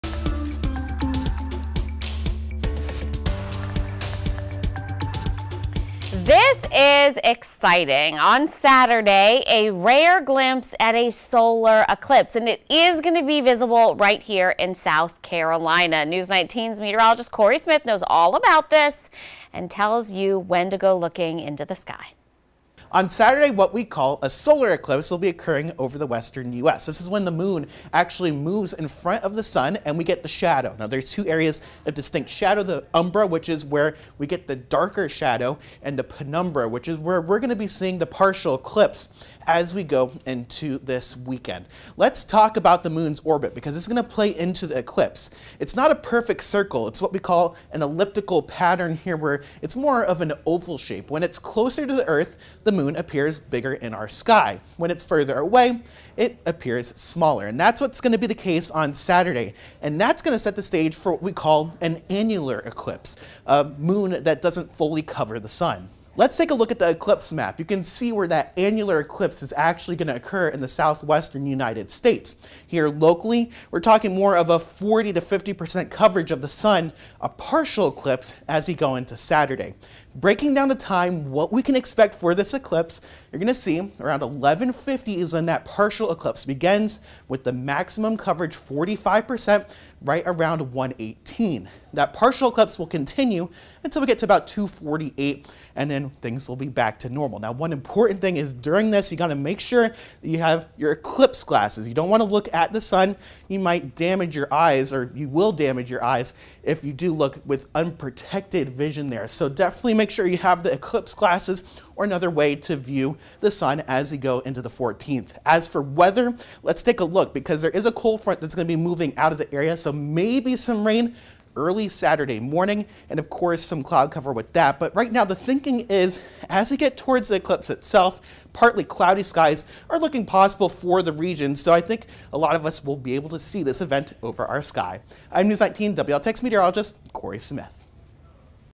Annular Solar Eclipse in South Carolina: What you can expect to see
An exciting astronomical event will be taking place in our sky this weekend. The Moon will be passing over the sun in what is known as a solar eclipse.